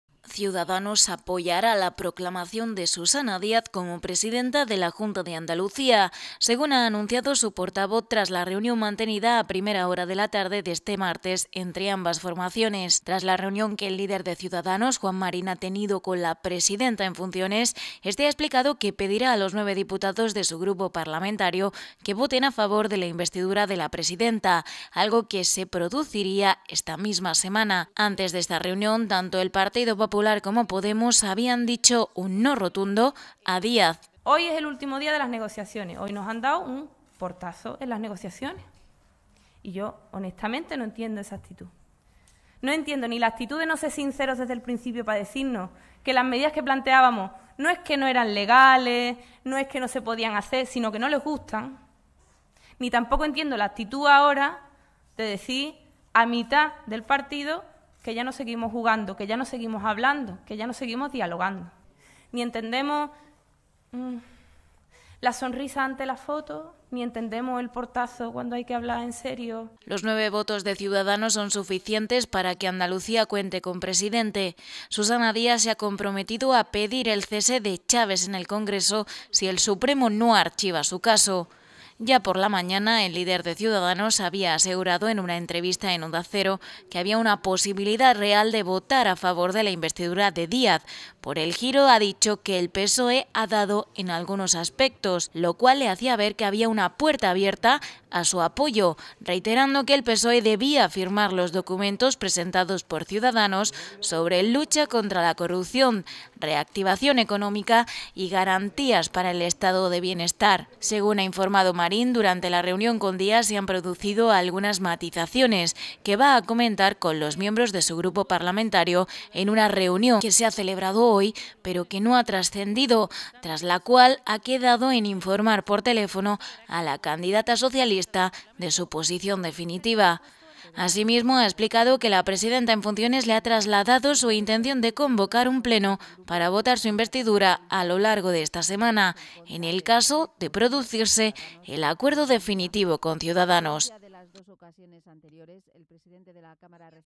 Los acuerdos de la Junta de la Andalucía.
Ciudadanos votará 'sí' a la investidura de Susana Díaz esta misma semana. La presidenta en funciones logra el apoyo de la formación naranja, suficiente para ser proclamada.